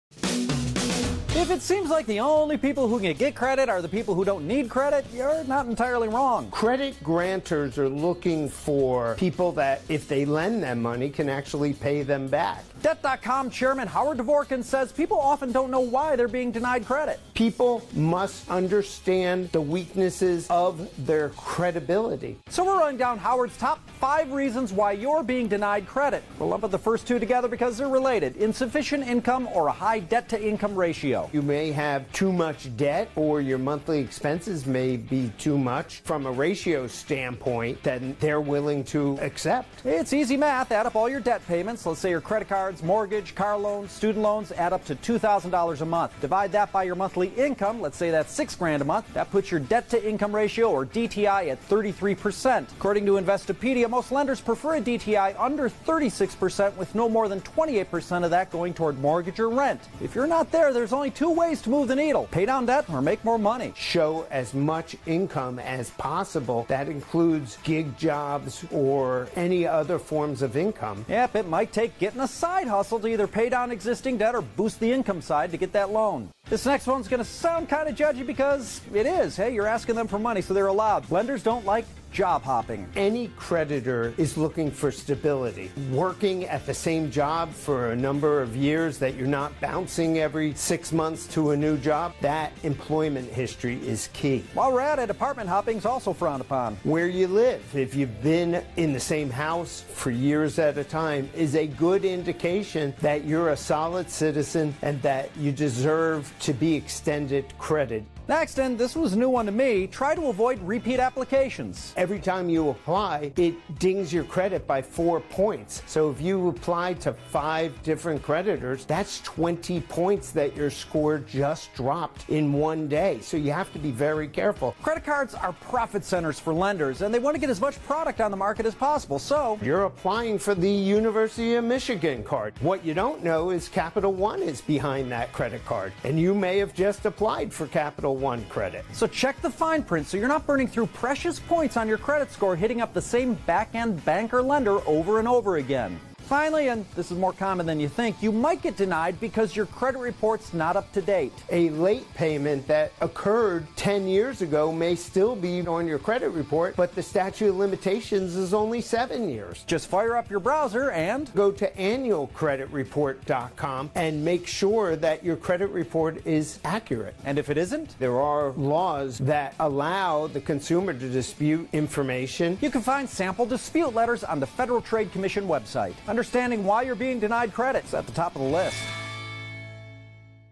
The List: Why You Get Denied Credit
If it seems like the only people who can get credit are people who don't need credit, you're not entirely wrong.